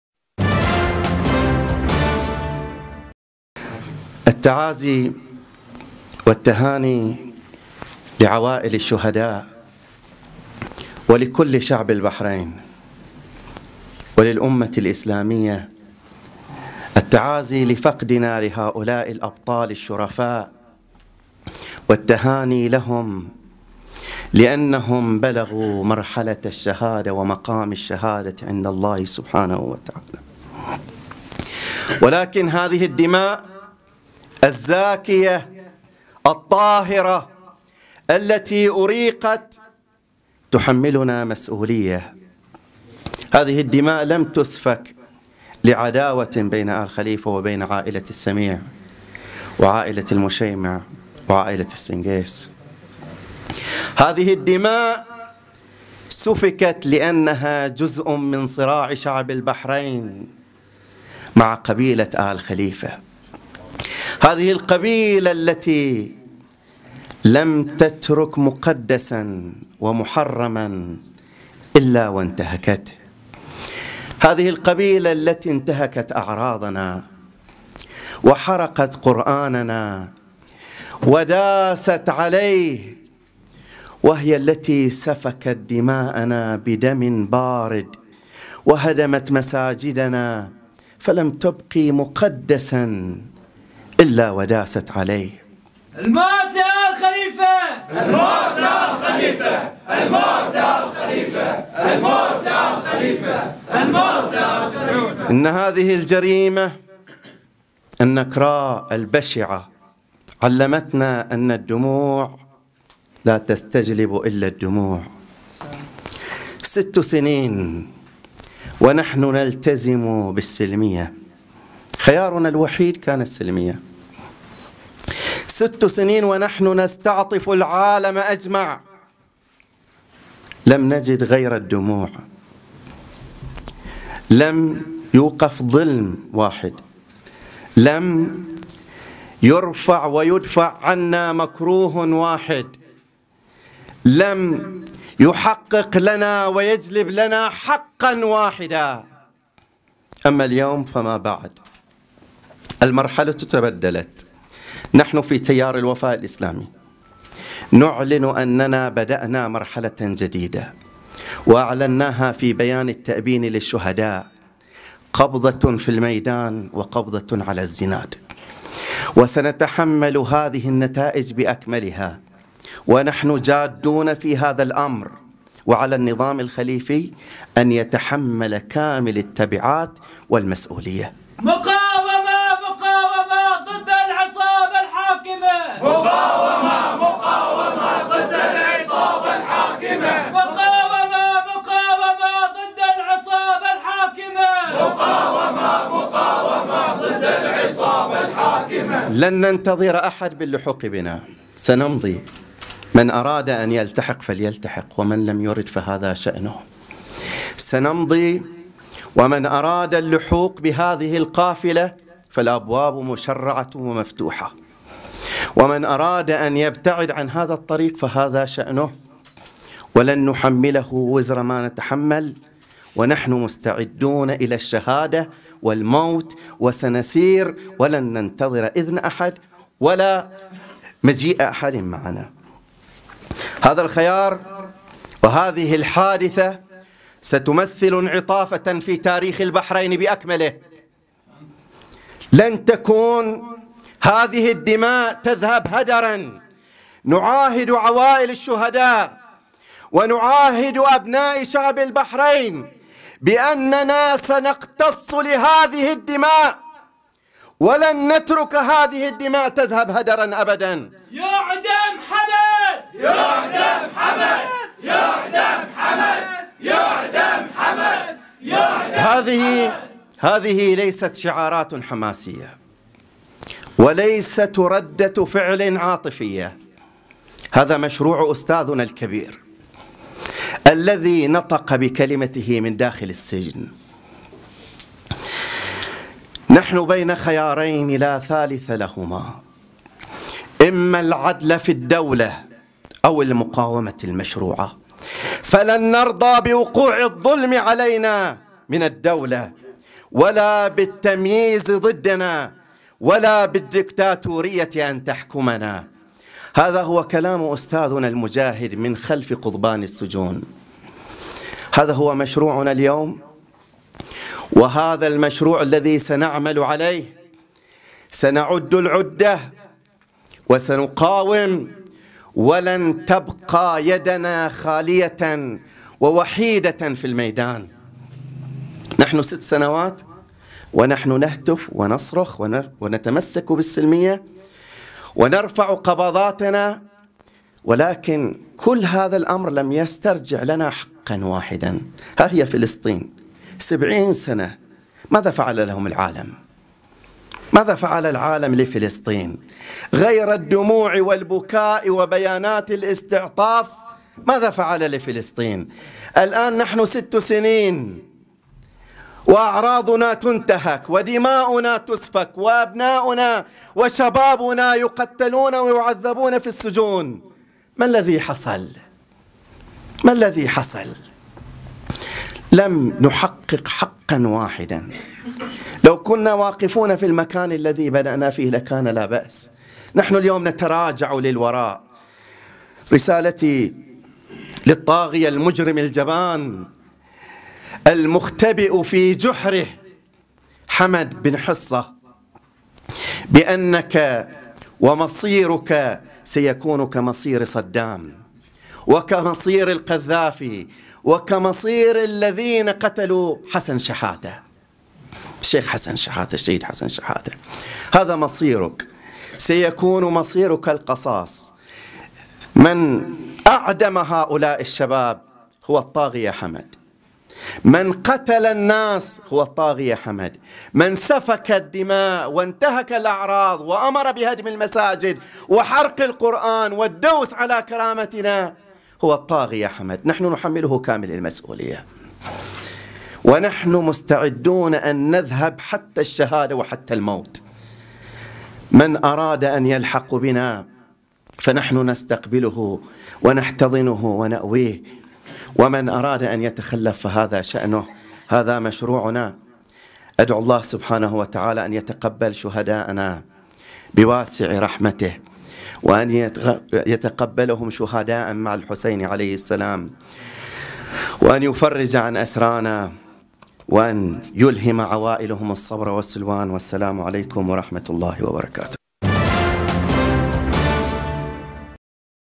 كلمة السيد مرتضى السندي كاملة حول إعلان الكفاح المسلح في البحرين 16/1/2017
كلمة الشيخ عبدالله الصالح - تأبين شهداء البحرين المعدومين - قم المقدسة 16/1/2017